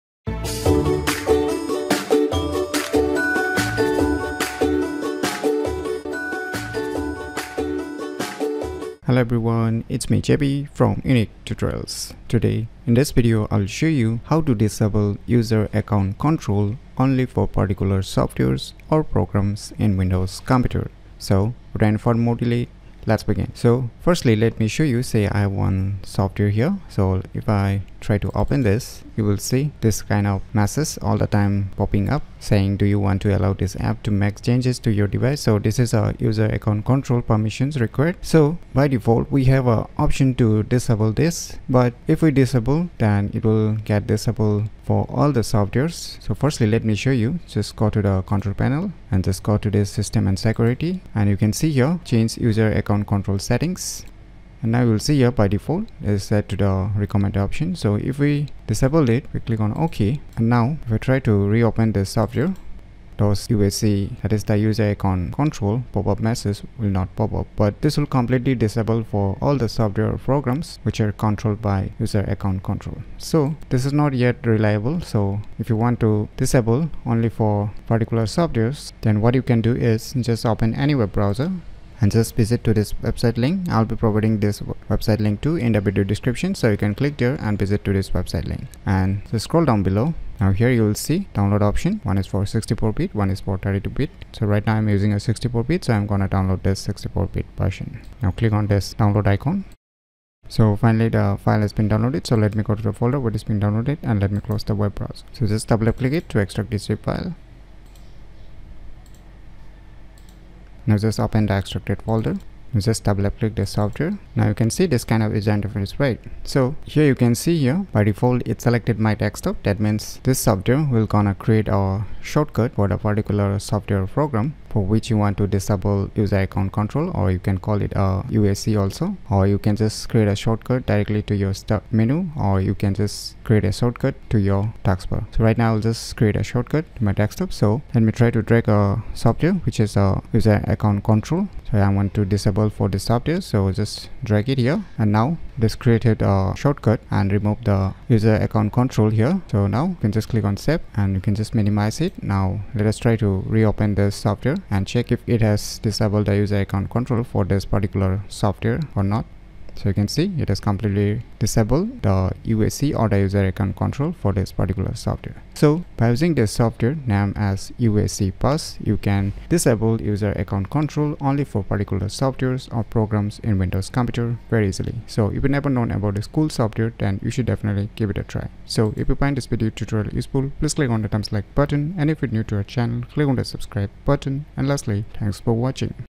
How to Disable User Account Control only for particular softwares or programs in Windows computer ?
This video tutorial is all about How to Disable User Account Control only for particular softwares or programs in Windows computer.